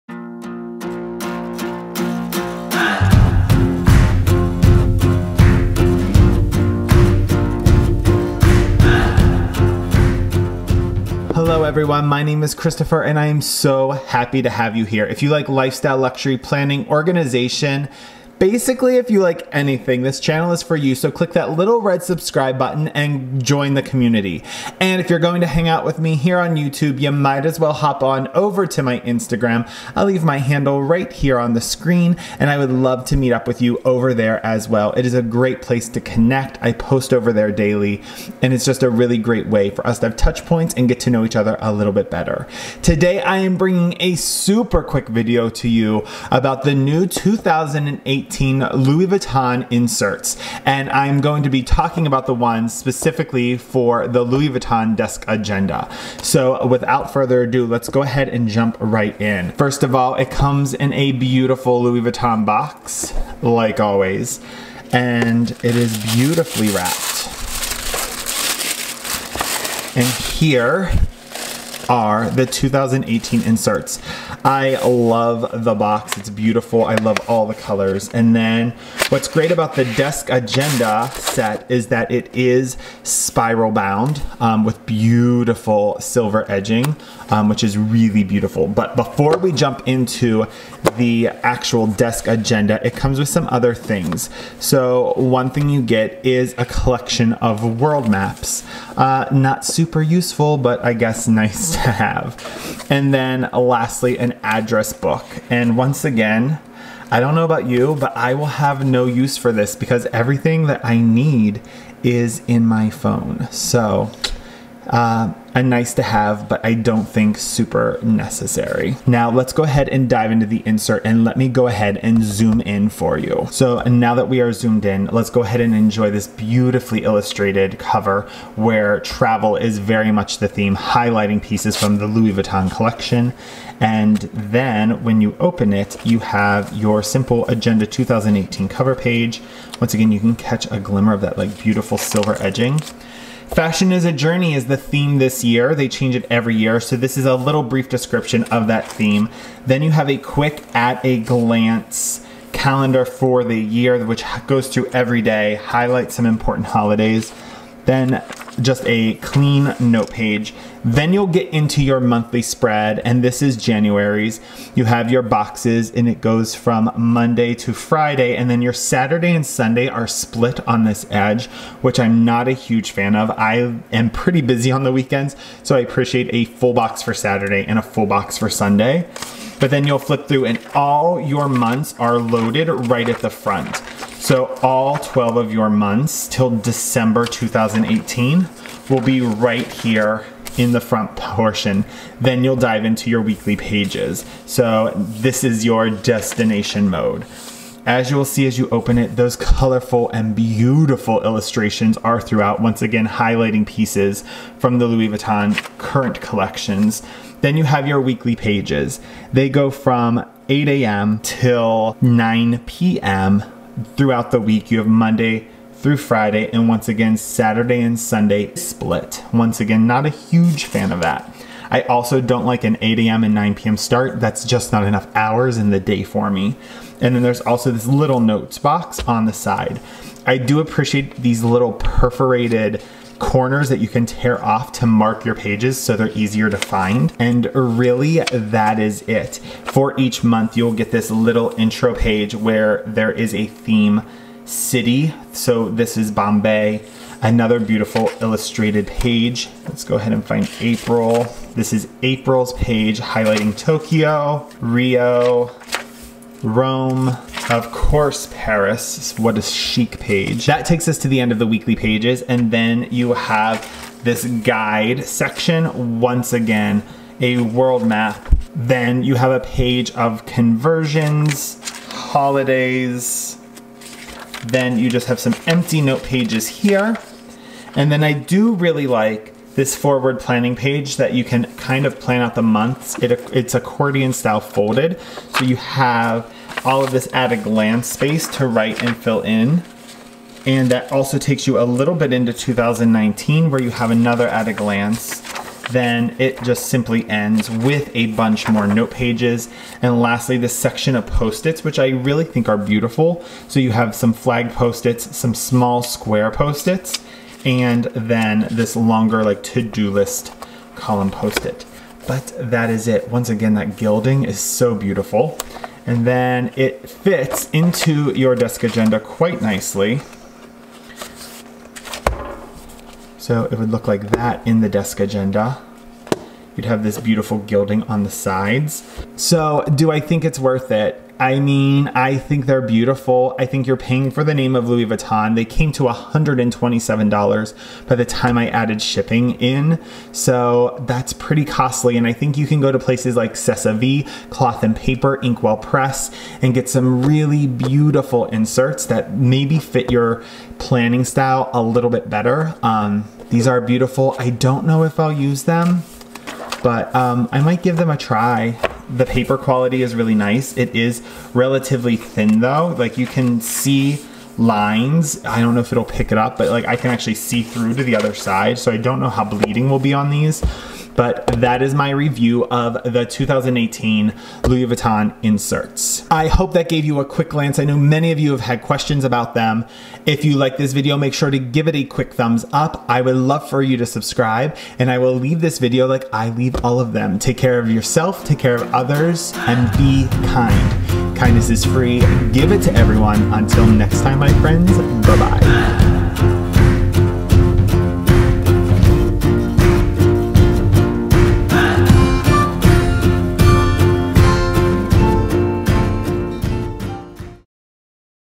2018 Louis Vuitton Agenda Insert Review
Hello all! I went ahead and purchased the 2018 Louis Vuitton agenda inserts because several asked for my review on them. Here is it!

FOLLLOW ME!